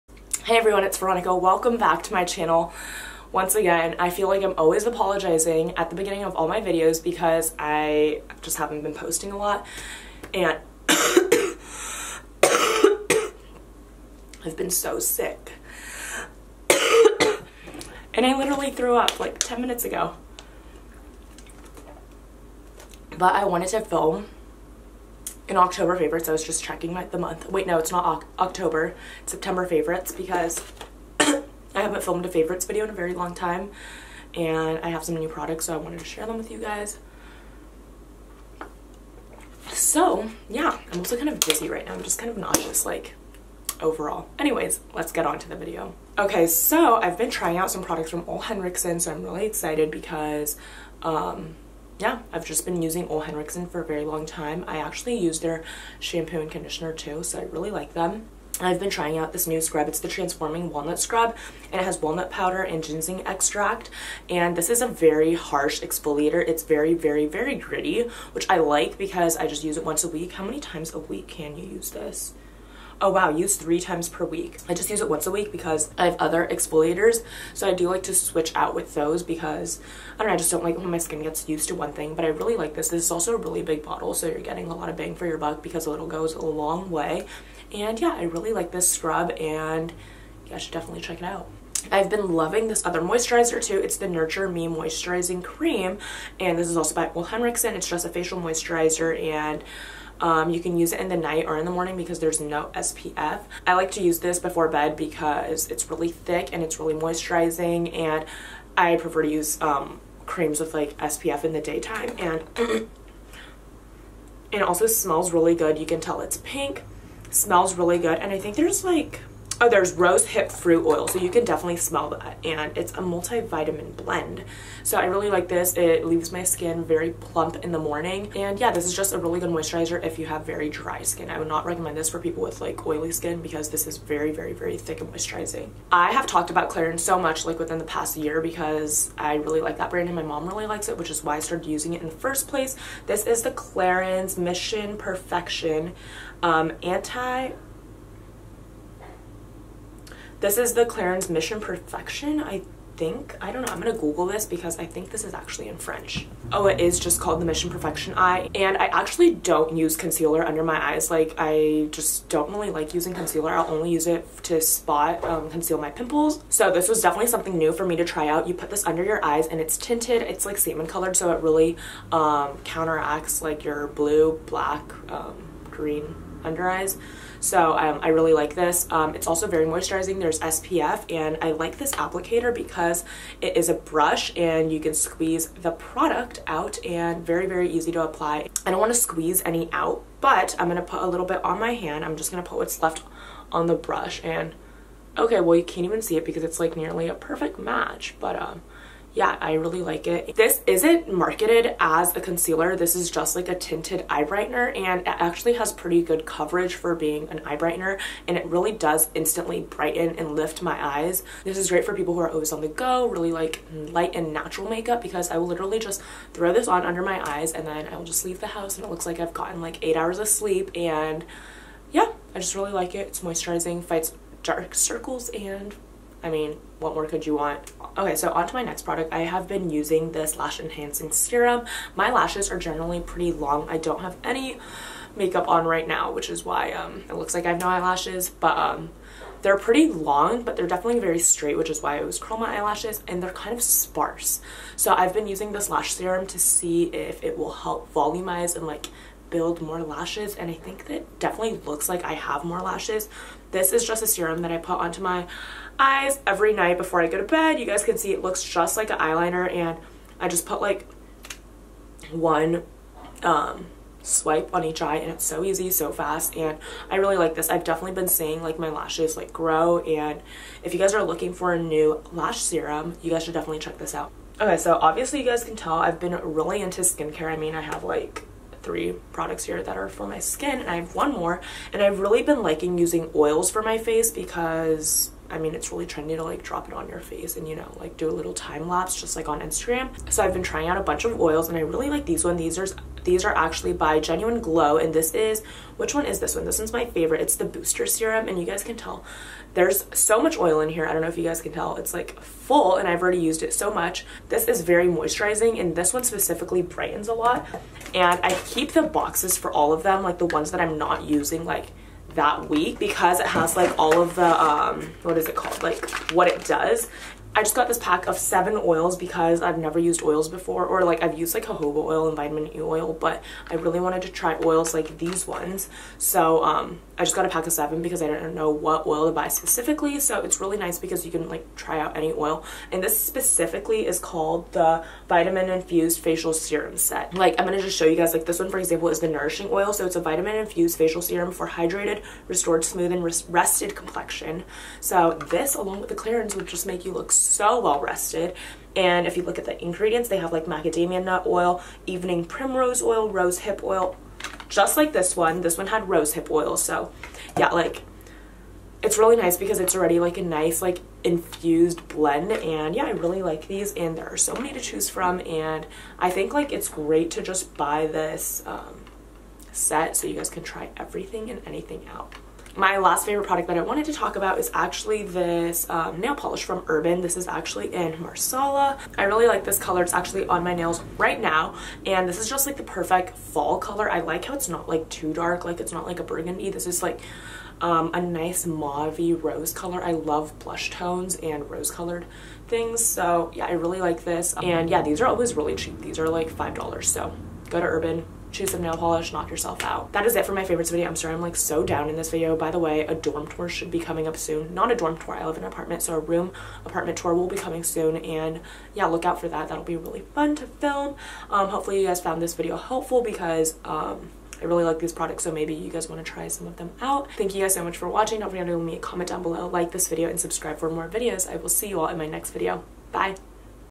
September Favorites 2017
Hey everyone! I wanted to share some products that I have been loving in this past month!